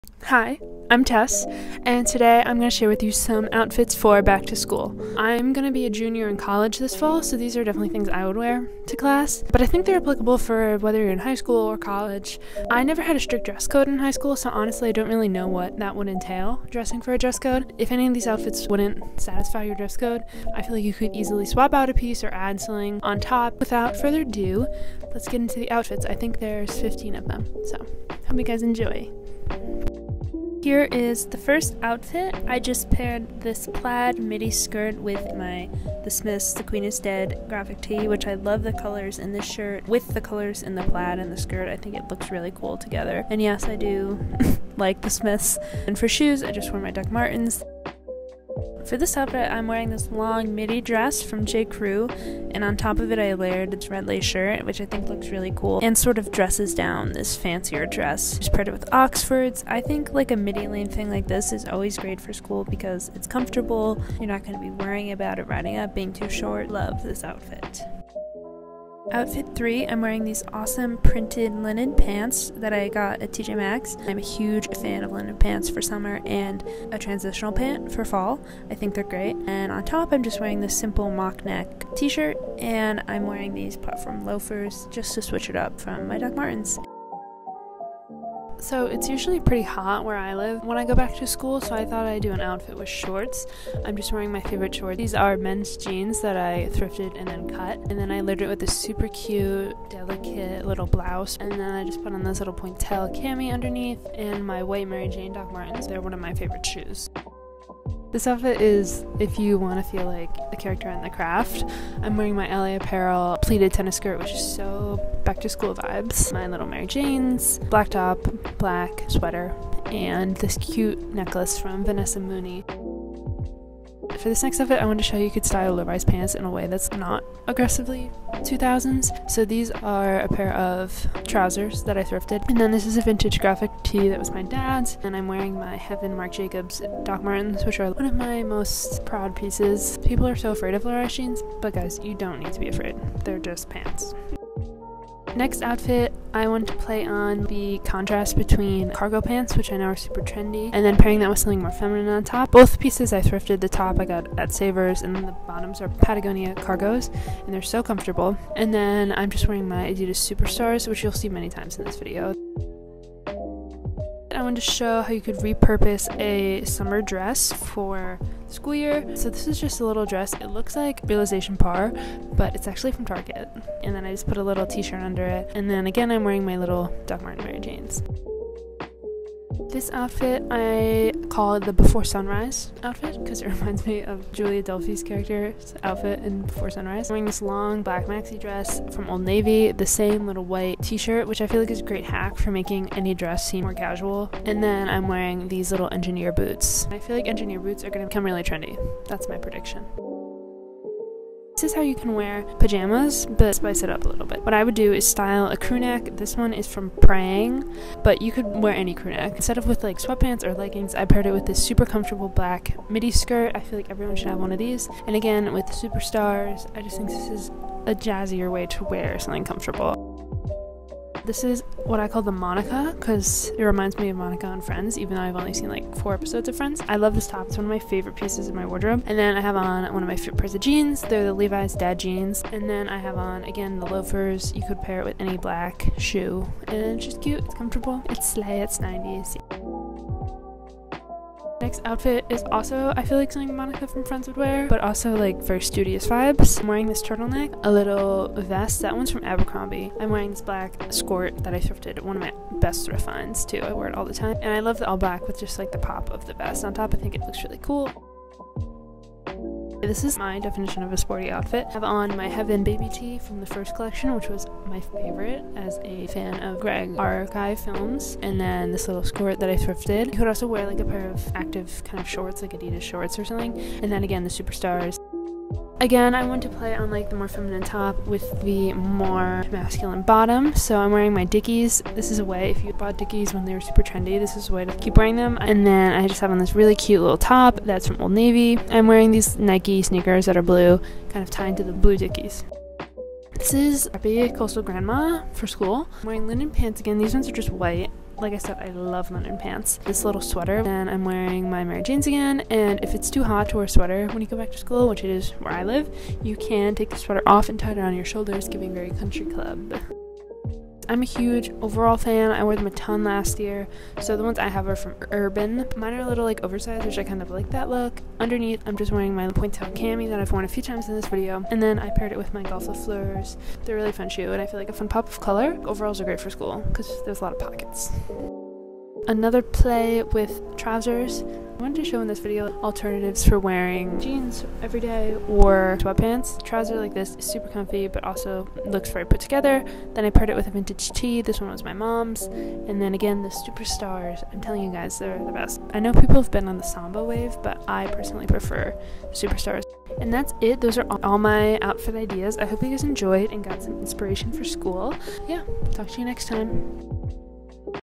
back to school outfit ideas
here are seventeen back to school outfit ideas from a junior in college.